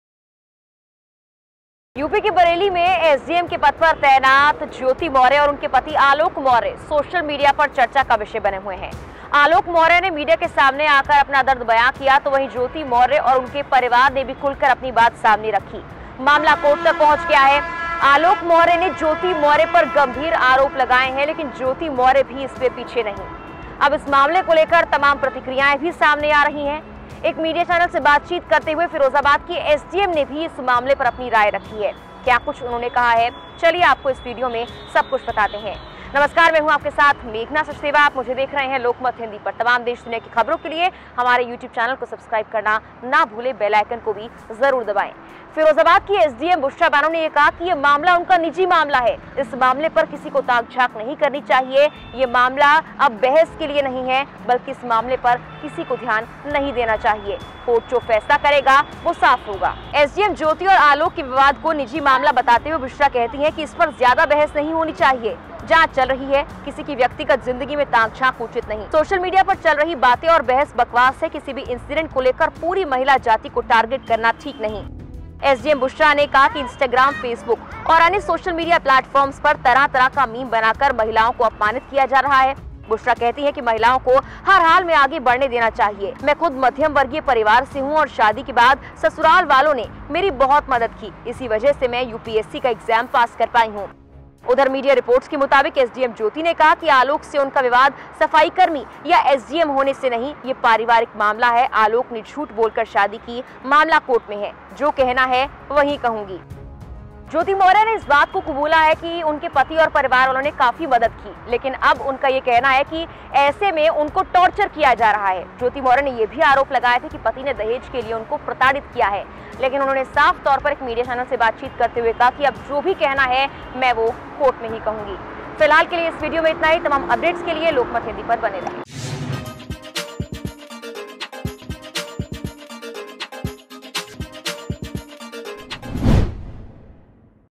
SDM Jyoti Maurya के विवाद पर क्या बोलीं फिरोजाबाद SDM Bushra Banu
एसडीएम ज्योति और आलोक के विवाद को निजी मामला बताते हुए बुशरा कहती हैं कि इस पर ज्यादा बहस नहीं होनी चाहिए